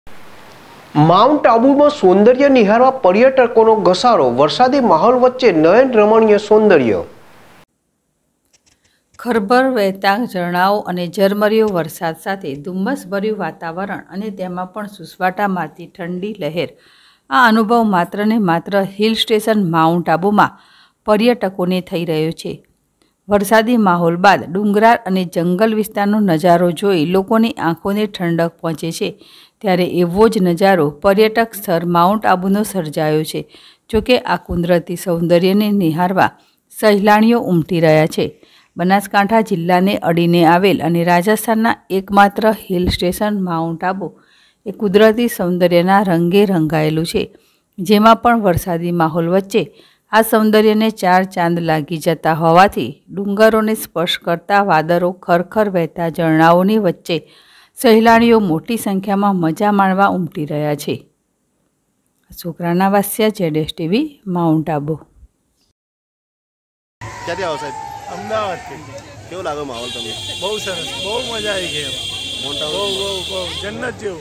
માઉન્ટ ના સૌંદર્ય નીહળવા પર્યટકો નો ઘસારોવરસાદી માહોલ વચ્ચે નયનરમ્ય સૌંદર્ય
માઉન્ટ ના સૌંદર્ય નીહળવા પર્યટકો નો ઘસારો

વરસાદી માહોલ વચ્ચે નયનરમ્ય સૌંદર્ય